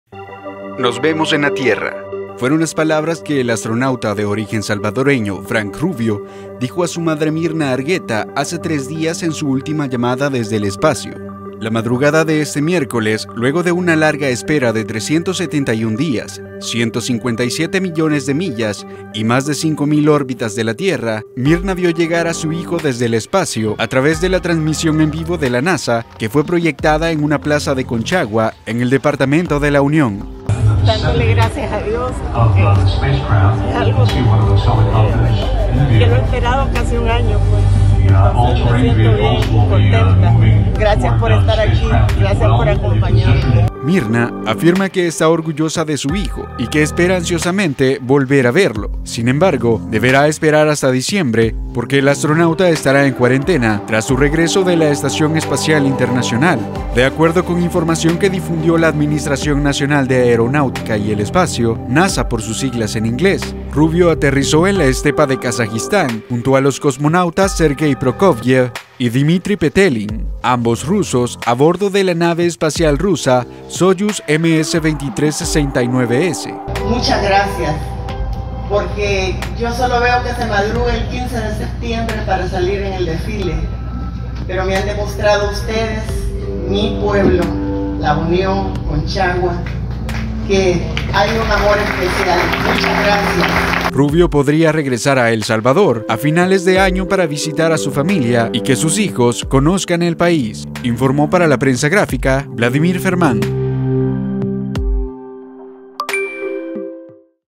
Así se vivió la llegada a la Tierra del astronauta Frank Rubio desde El Salvador
El astronauta Frank Rubio regresó a la Tierra después de permanecer más de un año en el espacio. Así se vivió su llegada desde El Salvador